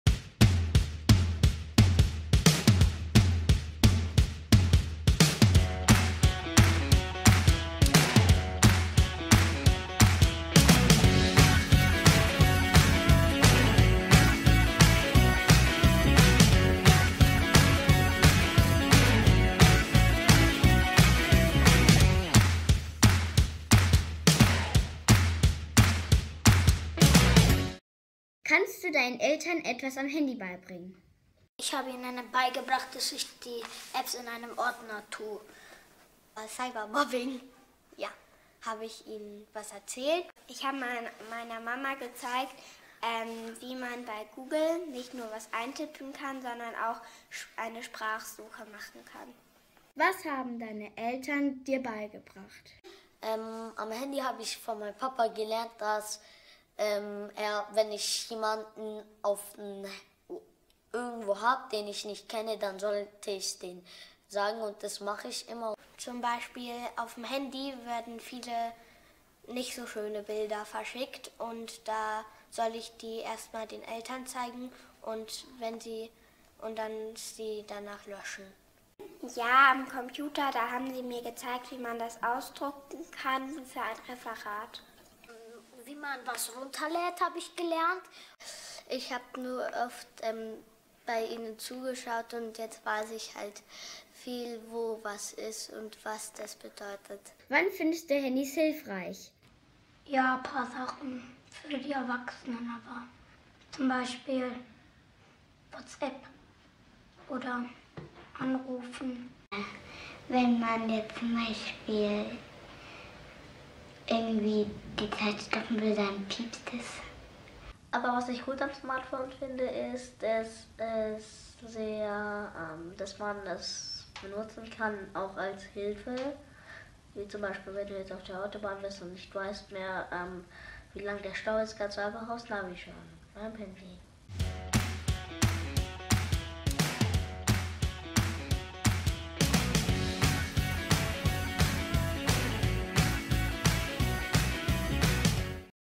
Liebe Eltern, hört mal her: Handys sind hilfreich (Clip 3 von 4)
Wir haben Kinder gefragt, wann sie Handys hilfreich finden und wann sie von der Handynutzung ihrer Eltern gestört sind.

Ein Projekt von SIN - Studio im Netz, mit freundlicher Unterstützung durch Itemo,
Vielen Dank an die beteiligten Kinder.